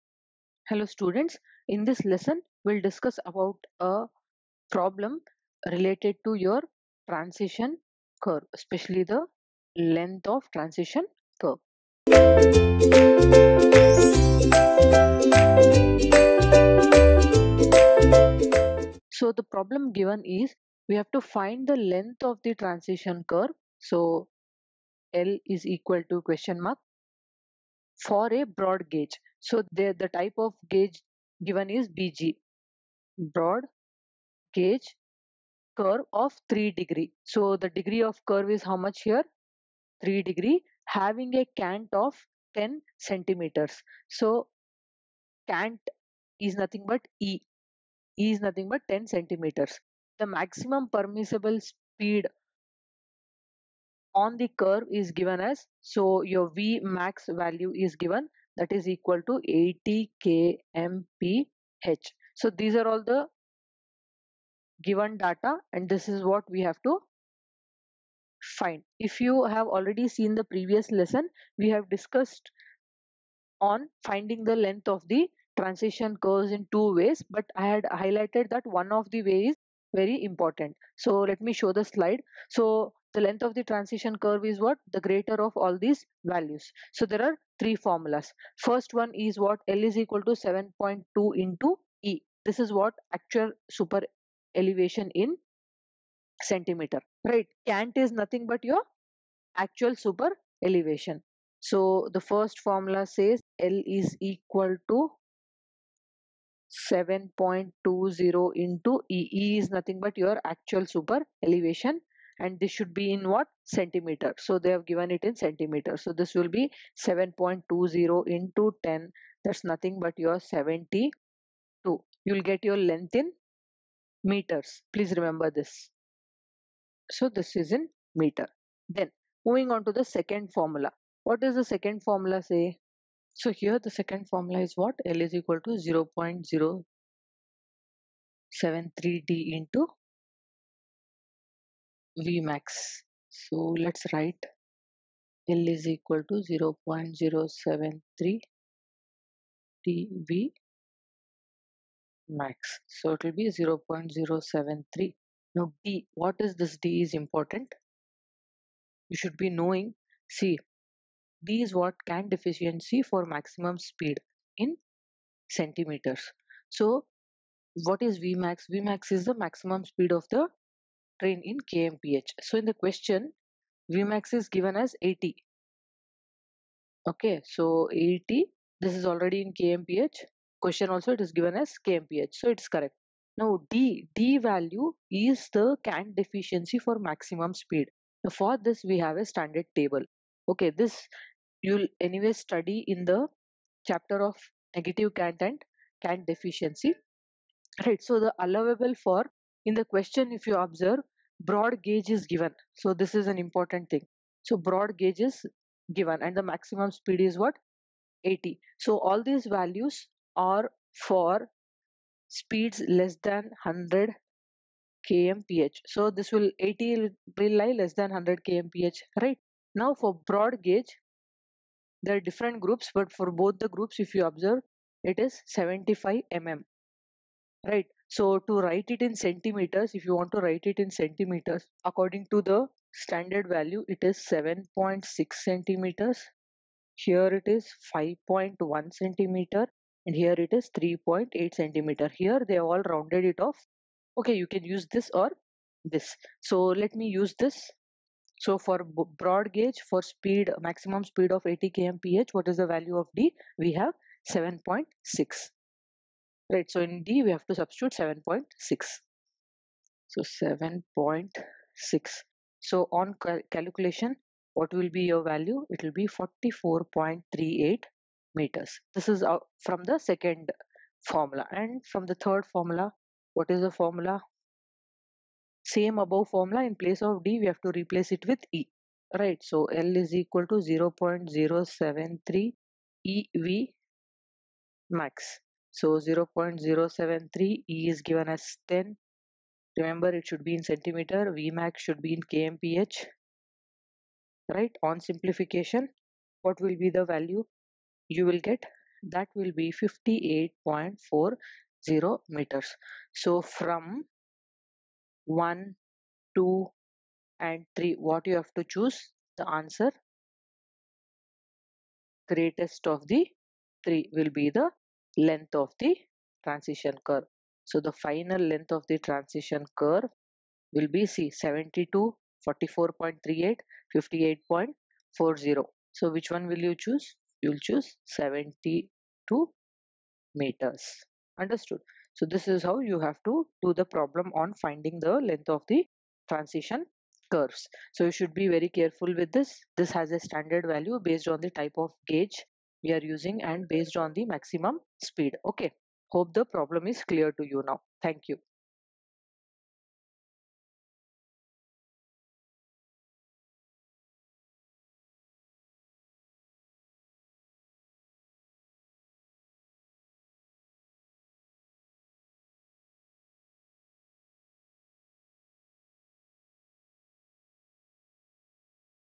Problem 1 - Length of Transition curves - Geometric Design of Railway - Transportation Engineering 2
Subject - Transportation Engineering 2

Video Name - Problem 1 - Length of Transition curves

Chapter - Geometric Design of Railway and Traffic Control

Faculty - Prof. Zoya Fathima

Happy Learning